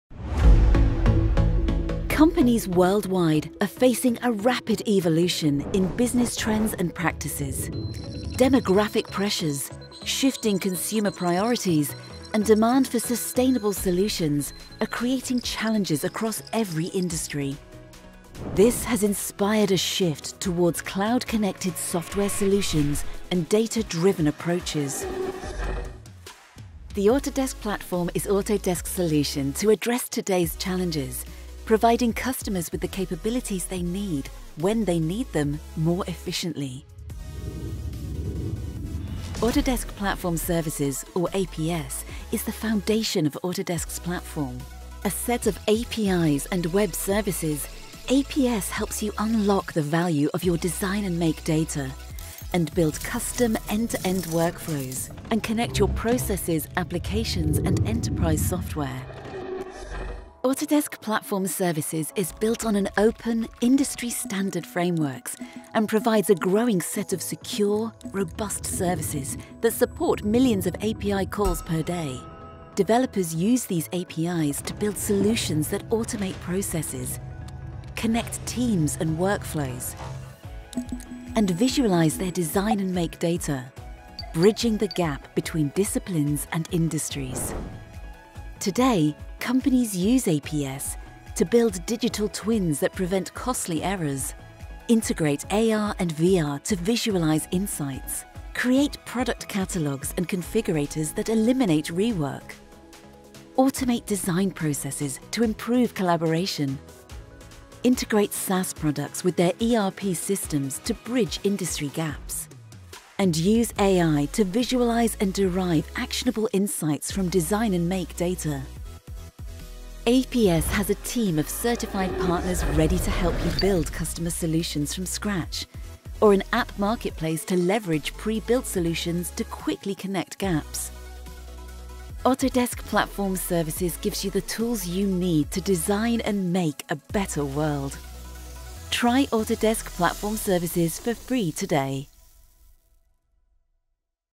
What is Autodesk Platform Services?
Autodesk Platform Services (APS) is the foundation of the Autodesk Platform. APS powers custom and pre-built applications, integrations, and innovative solutions. Our growing set of APIs and services unlocks your Design and Make Data and connects workflows. Access our toolkit to build business solutions that span disciplines and industries, or visit the Autodesk App Store to buy ready-made solutions.